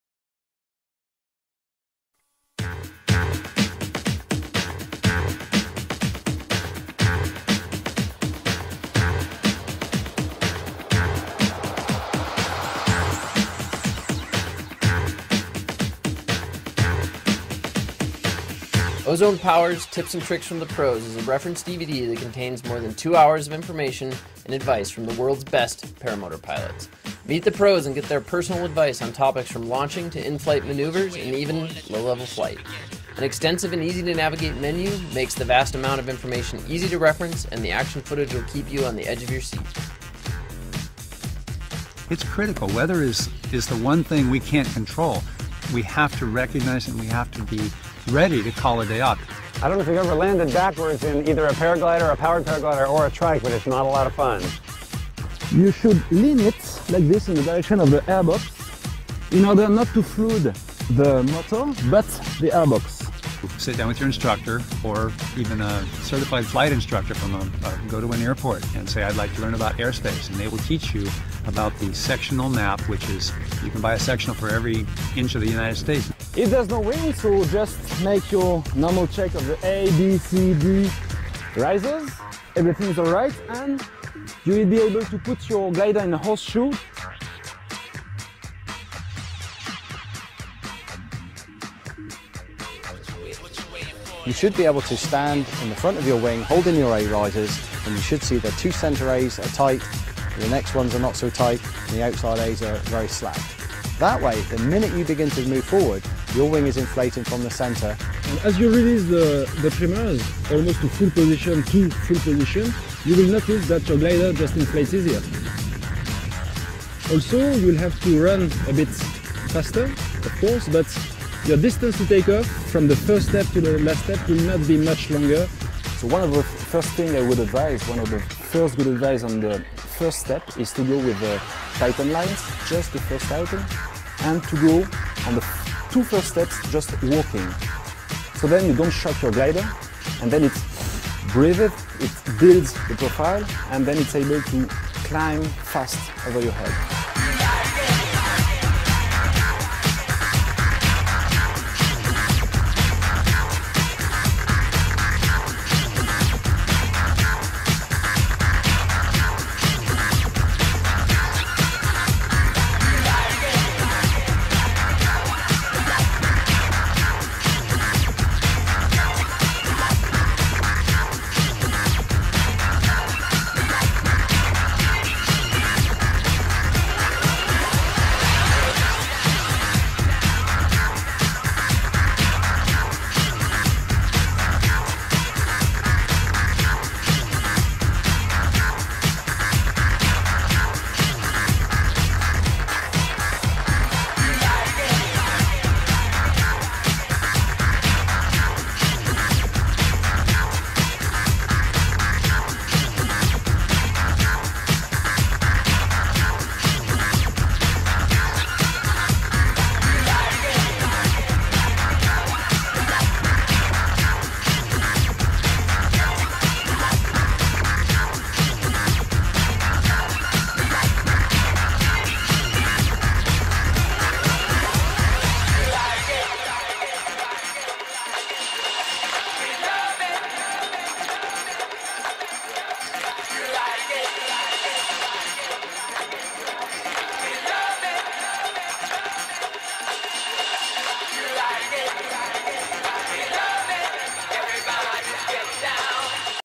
Ozone Paramotor PPG DVD tips and tricks _Trailer.flv
This is the latest Ozone DVD available in English only. It has load of really helpful tips and trips from David Dagult Mathieu Rouanet Squash Falconer and many other PPG intructors and Paramotorists.